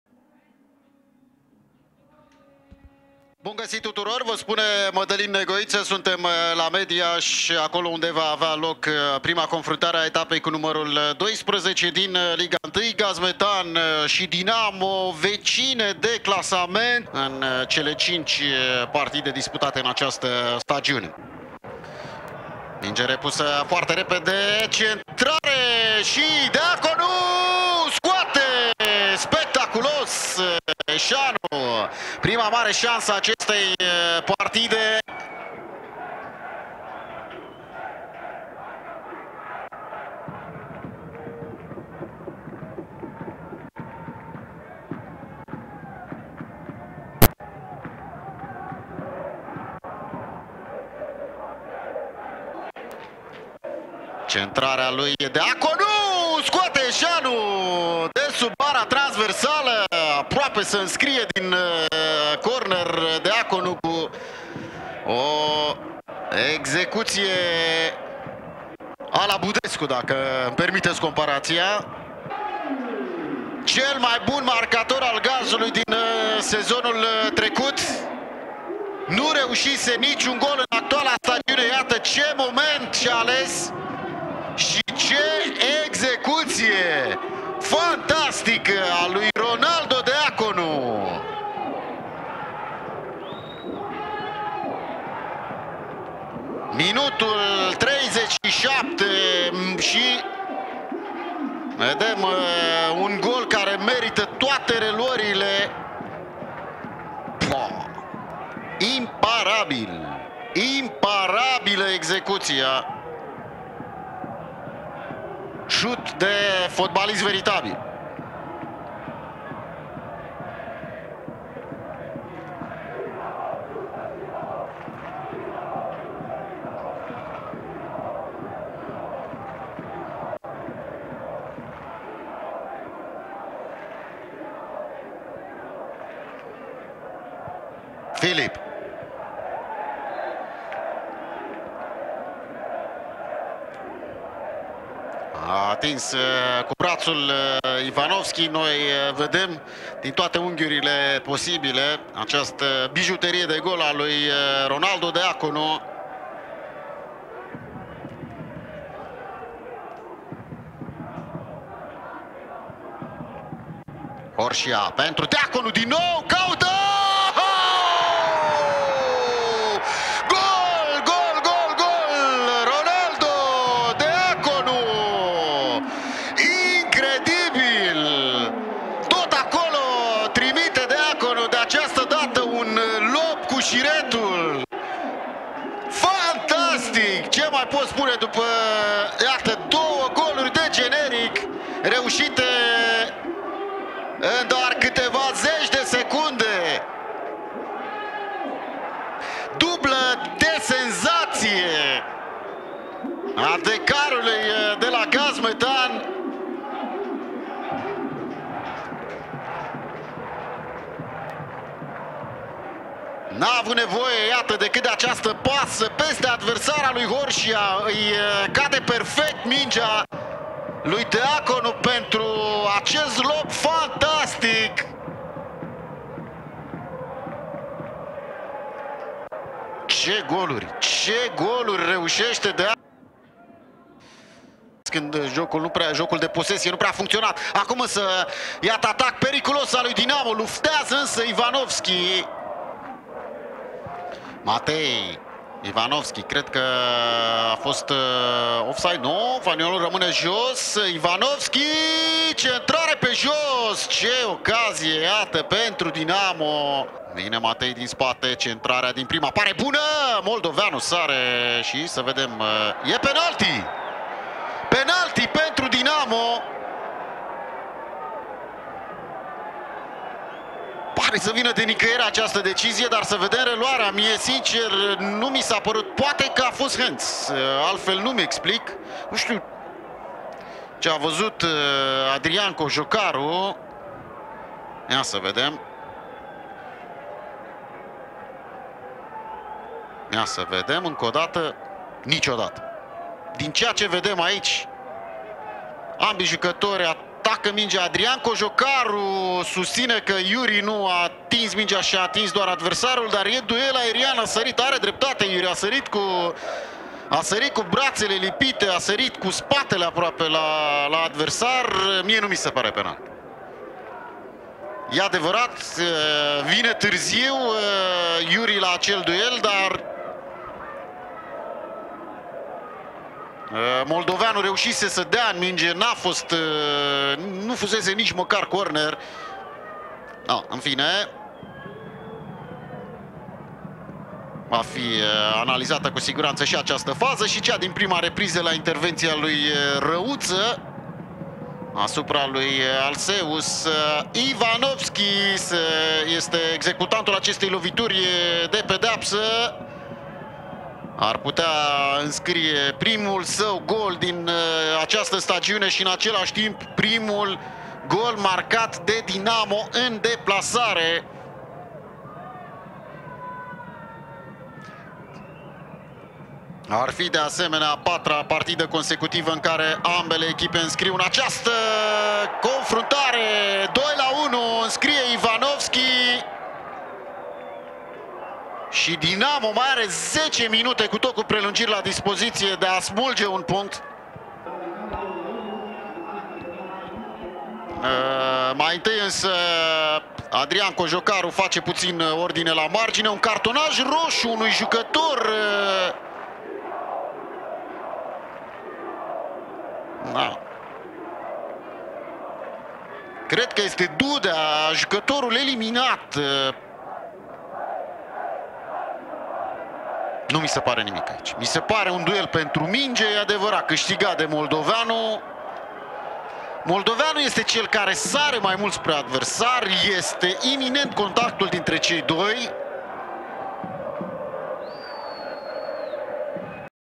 REZUMAT | Gaz Metan - Dinamo 2-1. Goluri senzaționale Deaconu, prima înfrângere pentru Rednic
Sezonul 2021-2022

Etapa a 12-a

Au marcat: R. Deaconu 36, 38 / Ivanovski 84 pen.

Echipele de start:

Gaz Metan: Vâlceanu - Moura, Matricardi, Yuri, Trif - Alceus, F. Junior - Horşia, Deaconu, Grădinaru - Sarr

Rezerve: Buzbuchi - Velisar, Horj, Filip, Şteau, Chamed, Morar, Irobiso, Pereira

Dinamo: Eşanu - Creţu, Pinto, Popa, Filip - Itu, Răuţă, Matei - Sorescu, Ivanovski, Torje

Rezerve: Fara - Radu, Giaffer, Grigore, Amzar, Dudea, Moldoveanu, Măgureanu, Borduşanu